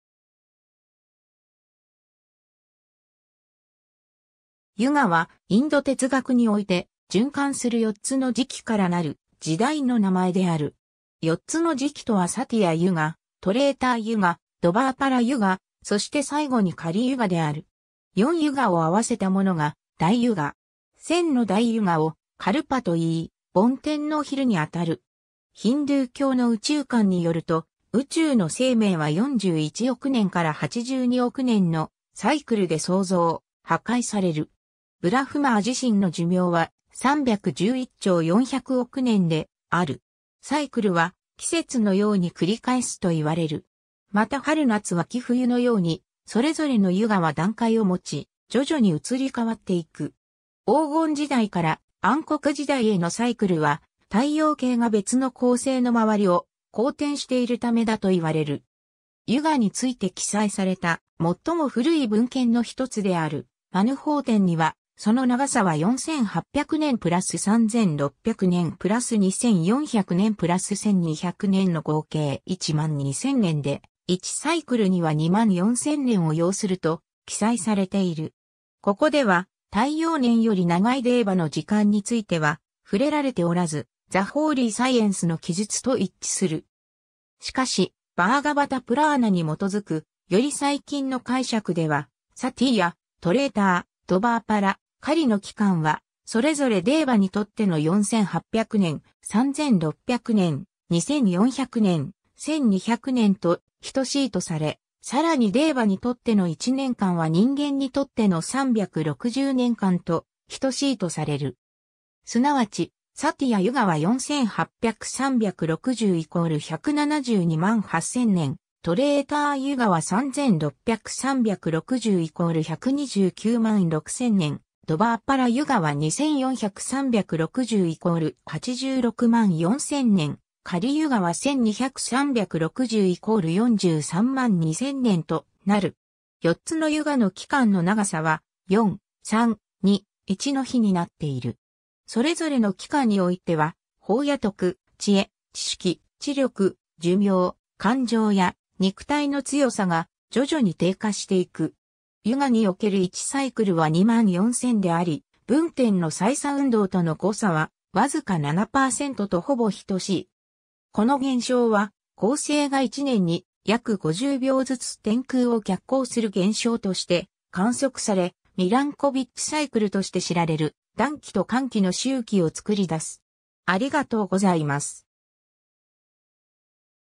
ユガ
ユガ(Yuga)は、インド哲学において、循環する4つの時期からなる「時代」の名前である。
4つの時期とはサティヤ・ユガ、トレーター・ユガ、ドヴァーパラ・ユガ、そして最後にカリ・ユガである。
4ユガを合わせたものが大ユガ（432万年）。
千の大ユガをカルパ（劫）といい、梵天（ブラフマー）の昼（夜）にあたる。
ヒンドゥー教の宇宙観によると、宇宙の生命は41億年から82億年のサイクルで創造、破壊される。
ブラフマー自身の寿命は、311兆400億年である。
サイクルは季節のように繰り返すと言われる。
また春夏秋冬のように、それぞれのユガは段階を持ち、徐々に移り変わって行く。
黄金時代から暗黒時代へのサイクルは、太陽系が別の恒星の回りを公転しているためだと言われる。
 ユガについて記載された最も古い文献の1つであるマヌ法典には、その長さは4,800年+3,600年+2,400年+1,200年の合計12,000年で、1サイクルには24,000年を要すると記載されている。
ここでは、太陽年より長いデーヴァの時間については触れられておらず、The Holy Scienceの記述と一致する。
しかし、バーガヴァタ・プラーナに基づく、より最近の解釈では、サティヤ、トレーター、ドヴァーパラ、カリの期間は、それぞれデーヴァにとっての4,800年、3,600年、2,400年、1,200年と等しいとされ、さらにデーヴァにとっての1年間は人間にとっての360年間と等しいとされる。
即ち、サティヤ・ユガは4,800×360=1,728,000年、トレーター・ユガは3,600×360=1,296,000年、ドヴァーパラ・ユガは2,400×360=864,000年、カリ・ユガは1,200×360=432,000年となる(Śrīmad Bhāgavatam 3.11.19) 。
4つのユガの期間の長さは、4:3:2:1の比になっている。
 それぞれの期間においては、法や徳、知恵、知識、知力、寿命、感情や肉体の強さが徐々に低下していく。
 ユガにおける1サイクルは24,000であり、分点の歳差運動との誤差はわずか7%とほぼ等しい。
この現象は恒星が1年に約50秒ずつ天空を逆行する現象として観測され、ミランコビッチ・サイクルとして知られる暖期と寒期の周期を作り出す。